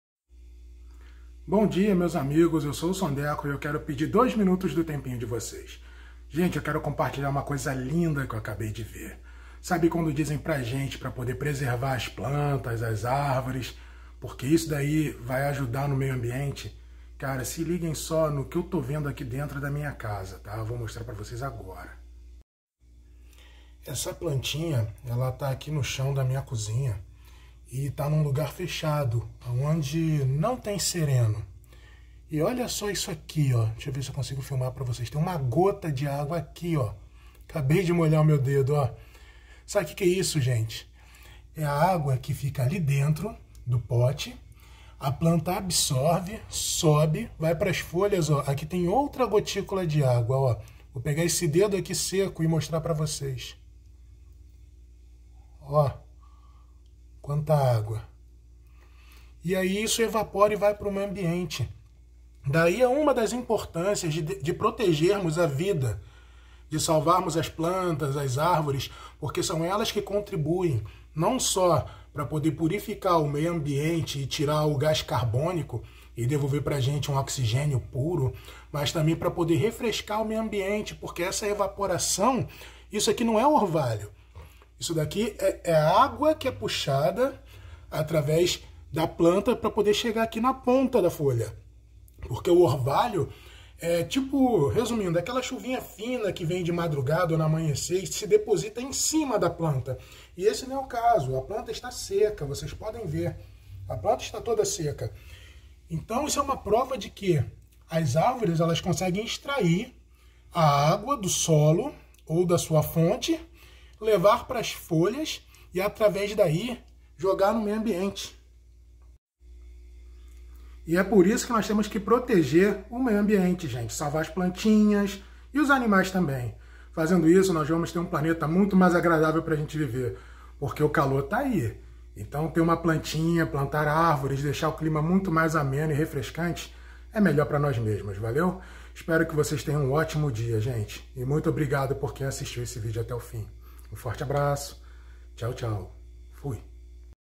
SALVEM A NATUREZA
Salvem as plantas e as árvores, elas refrescam o meio ambiente. Hoje eu vou mostrar como a natureza é bela e perfeita em seus pequenos detalhes.